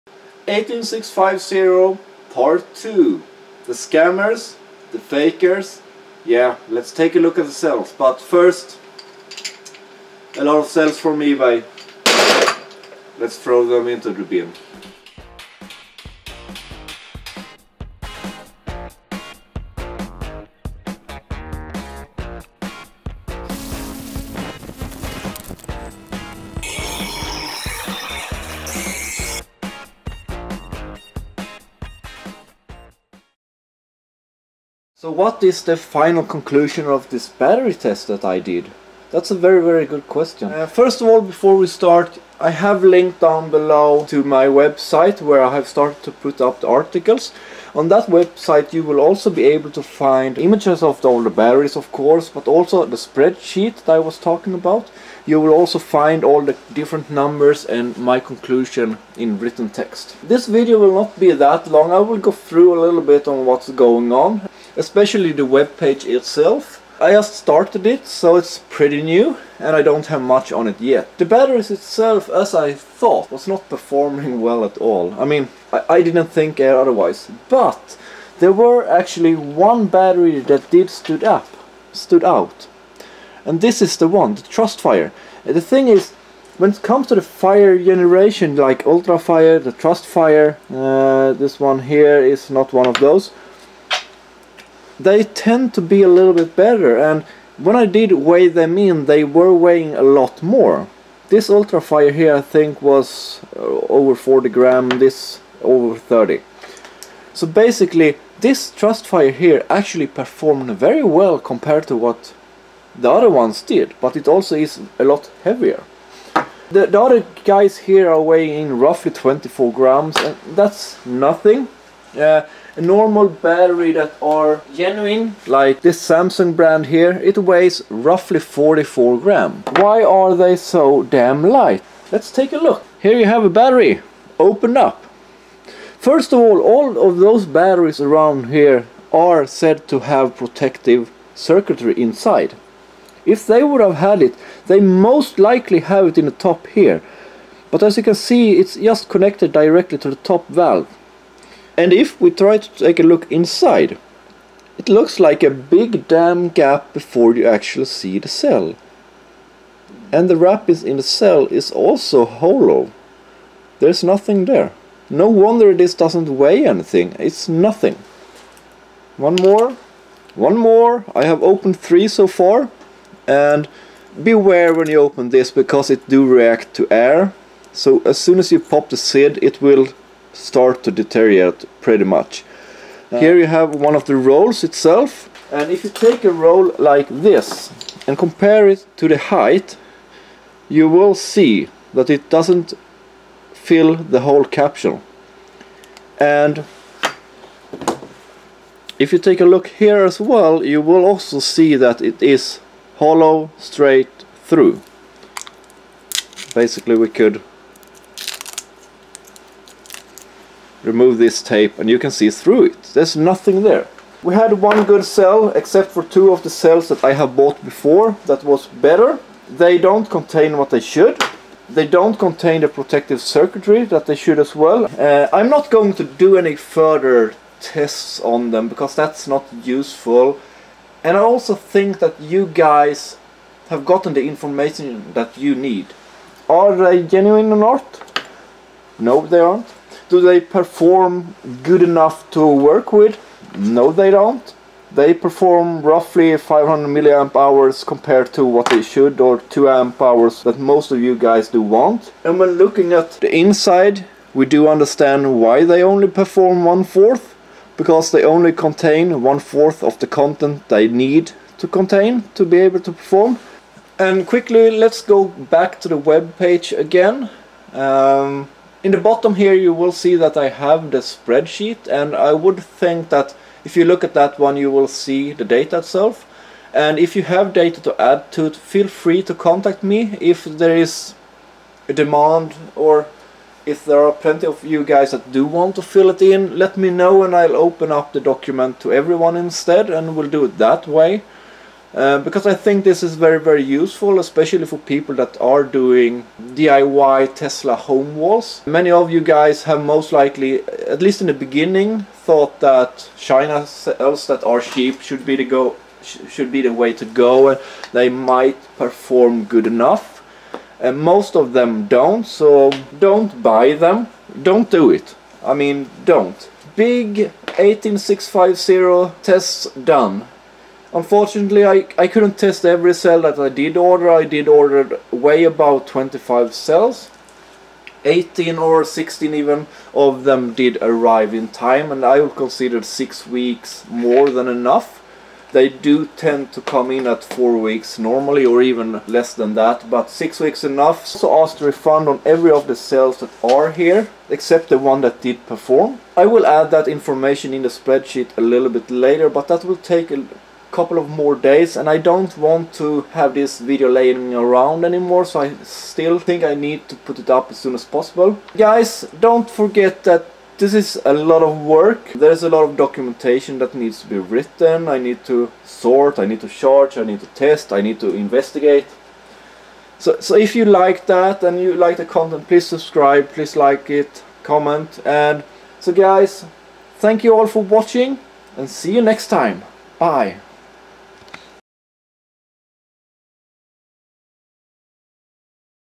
FAKE 18650 cells from fake sources (Part2)
If you find a source where the 18650 cells are cheap... Then its most likely to be fake. Especially if they aren't from genuine vendors.

In this video we will conclude the test and I show you what the result was.

Not surprisingly most cells did fail except for the more expensive one. But also what was interesting was that many of the ordered cells didn't even show up...

for more information and all the grafs from the tests!